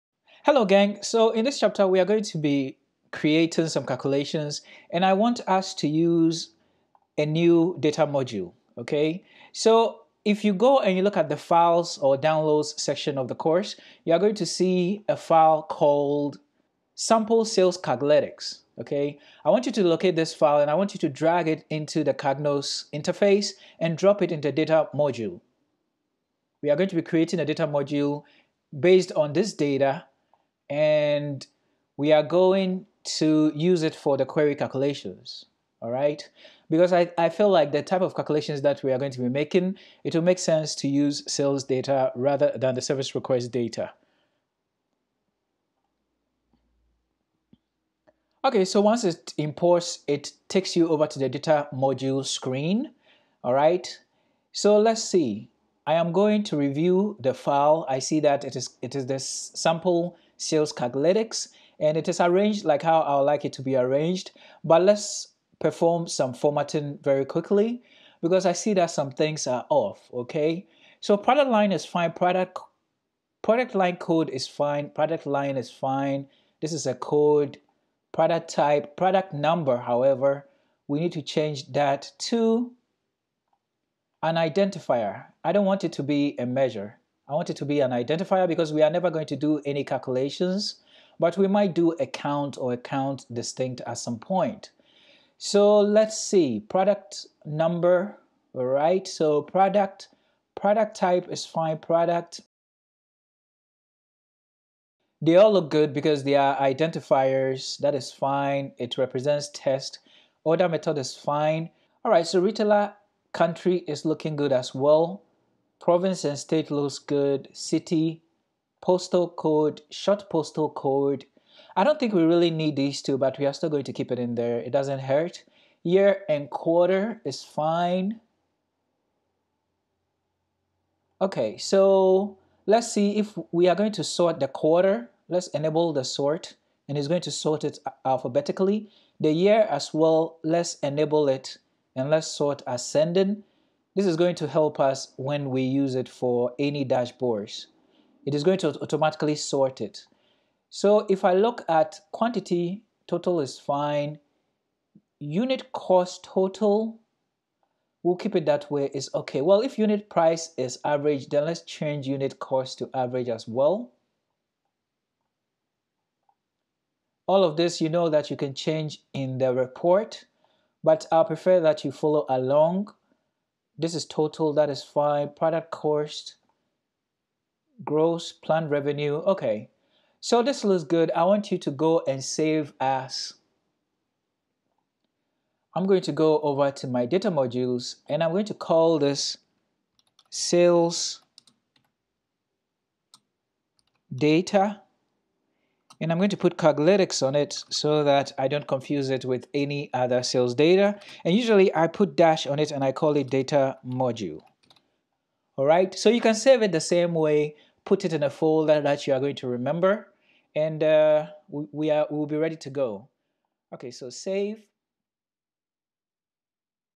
Create A Data Module in 5 minutes: Sample Sales Data For Data Analyst
In 5 minutes you can create a data module for your dashboards portfolio.